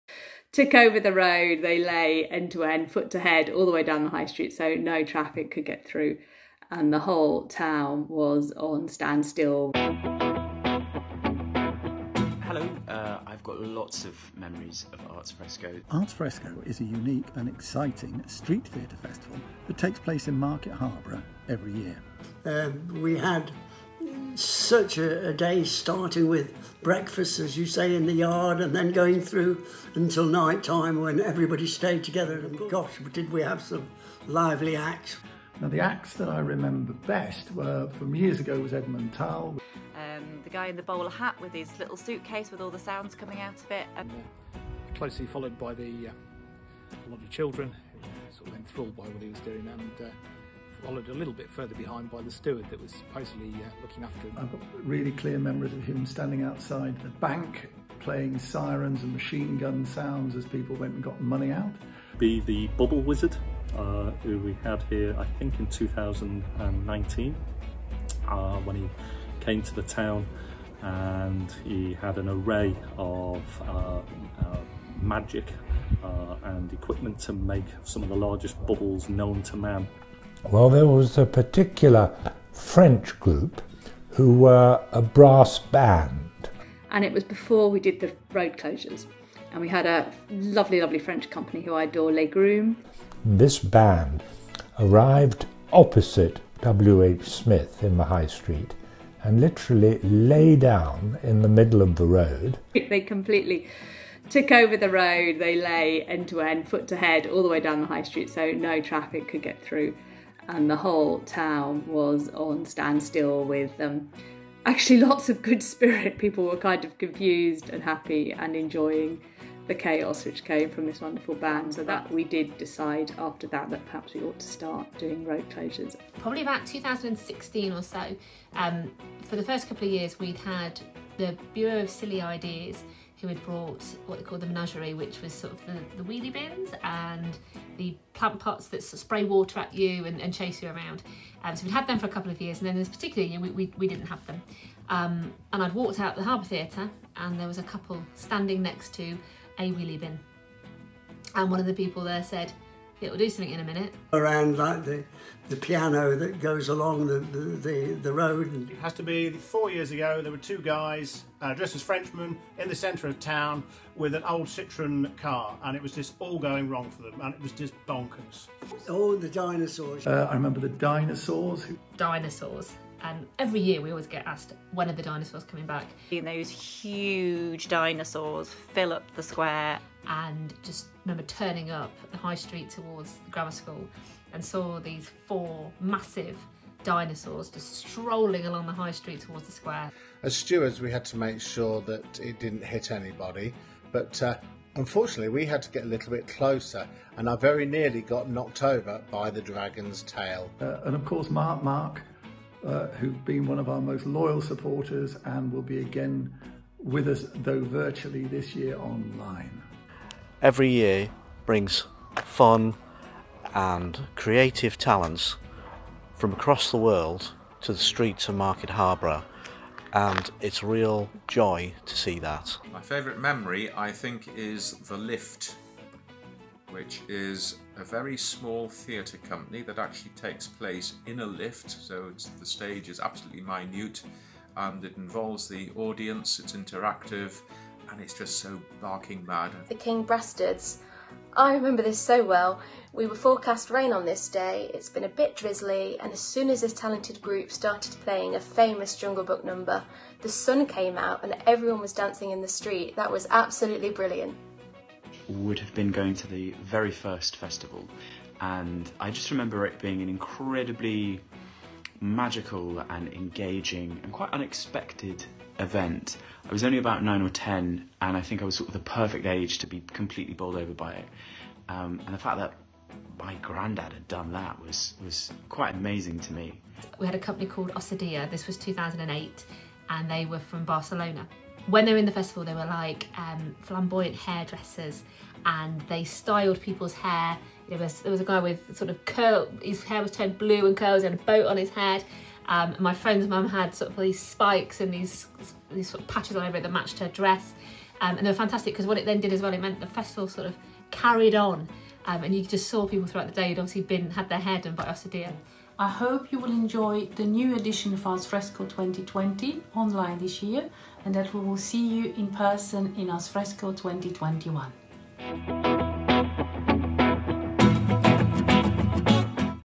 Memories of Arts Fresco
Celebrating UK street theatre at Arts Fresco 2020

Some of the people who have put together Arts Fresco over the years share some of their favourite memories. Will yours be in there too?

or follow on social media on Facebook, Twitter, Instagram and YouTube.